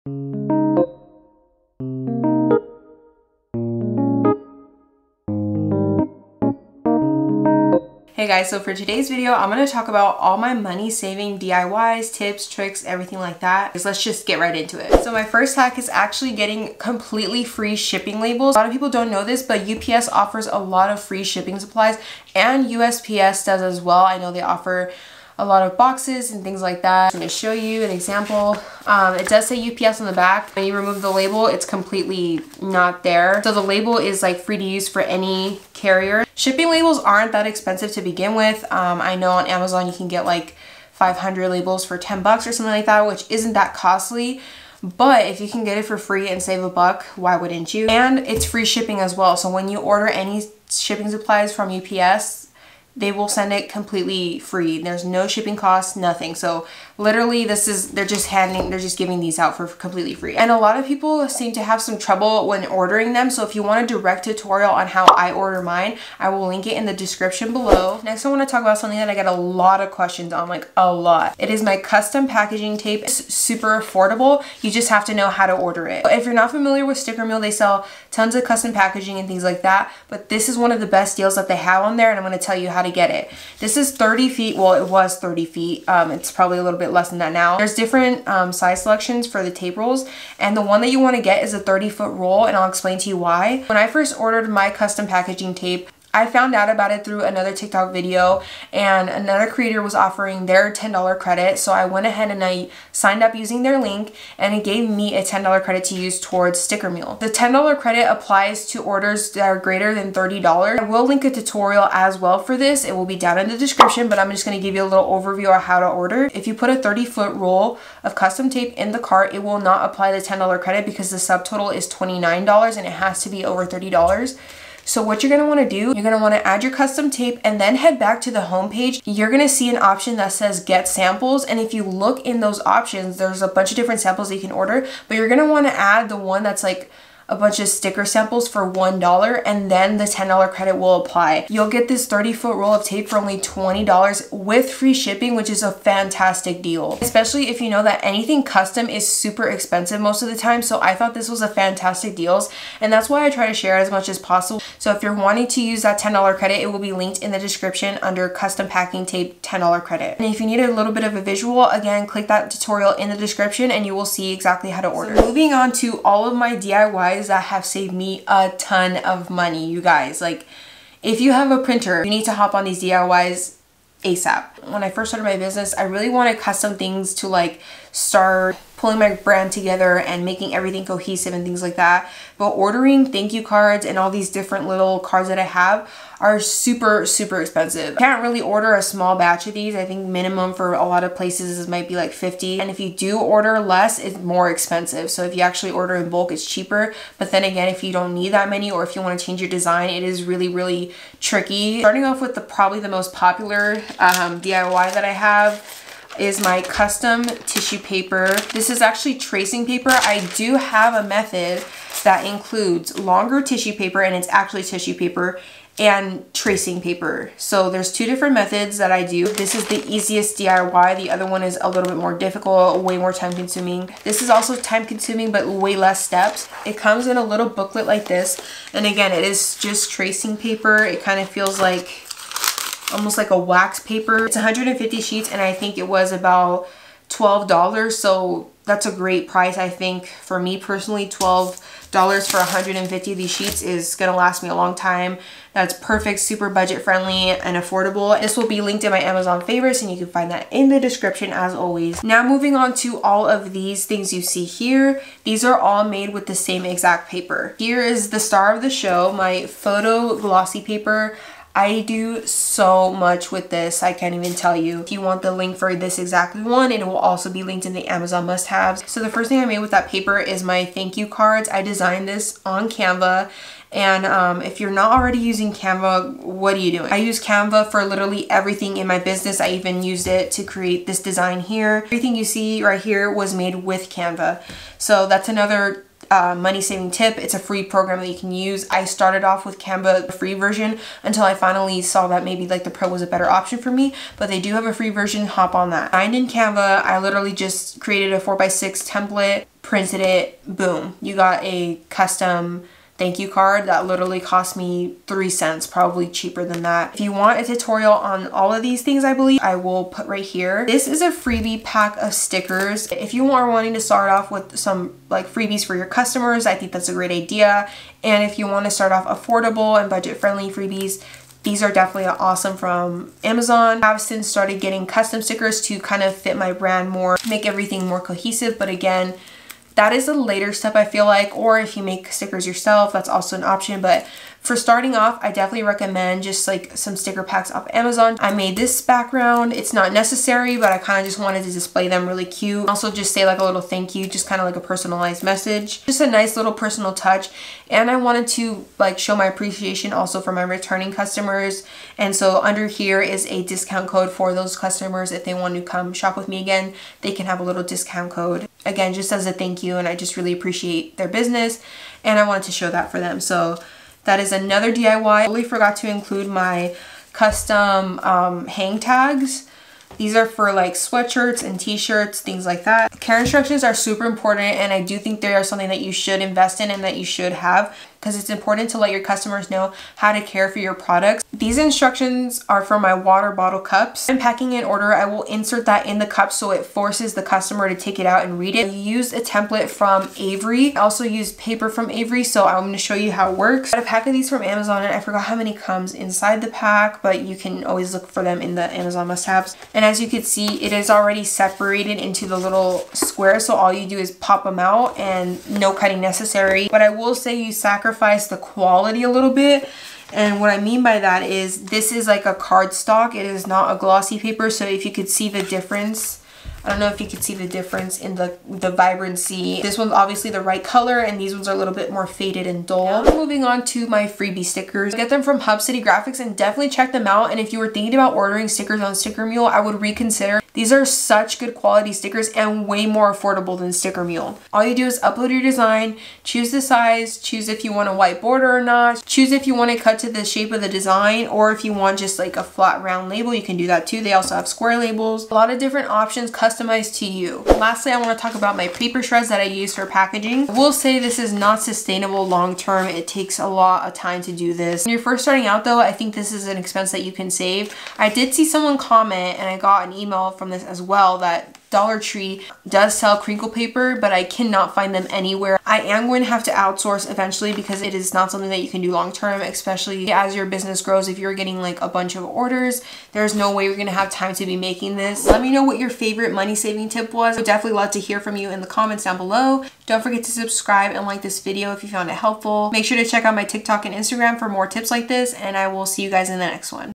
SMALL BUSINESS HACKS FOR SAVING MONEY |small business budget friendly packaging hacks|free packaging
Here are my top favorite small business hacks for saving money!

AMAZON FAVORITES

TUTORIALS:
UPS LABELS ORDERING

CUSTOM TISSUE PAPER TUTORIALS:
TRACING PAPER METHOD